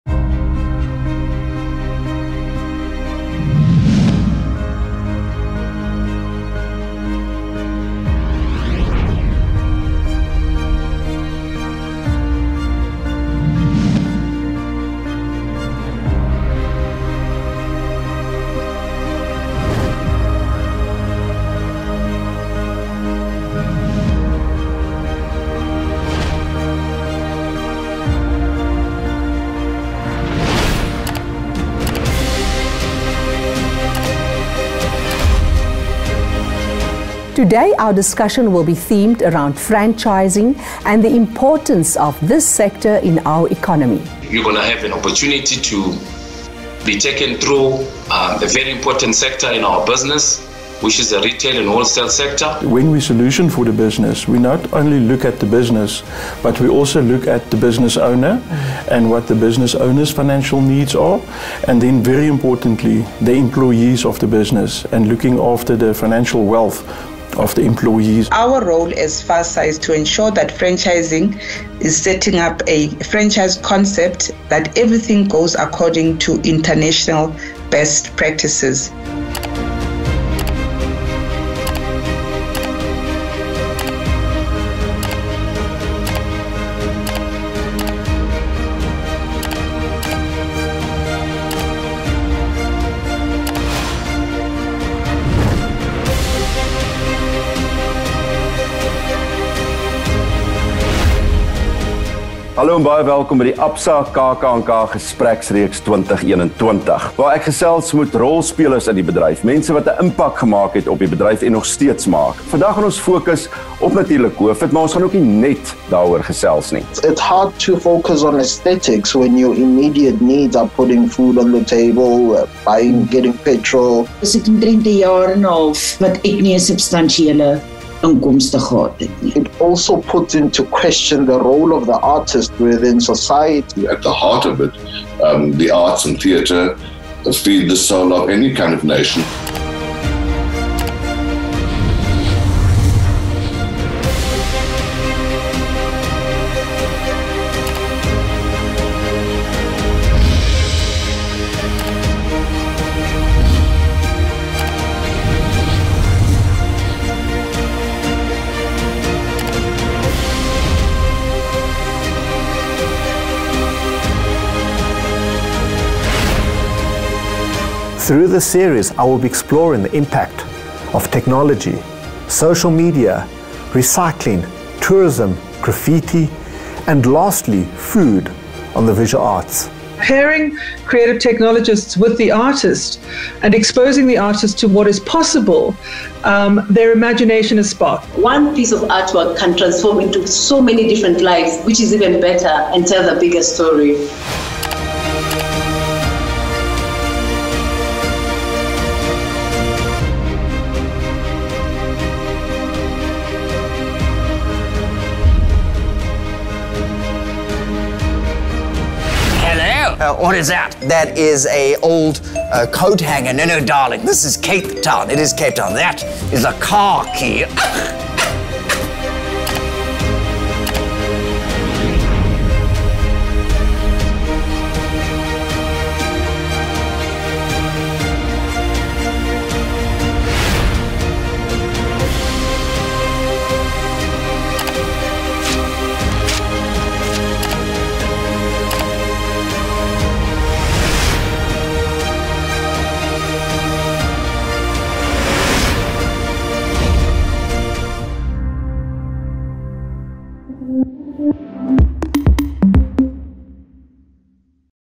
Experience the Absa Kuierkamer 2021 virtually
Whether from your living room, kitchen or even your bathroom, experience the Absa Kuierkamer at the KKNK 2021 from wherever you’re at. Here’s a highlight of what to expect.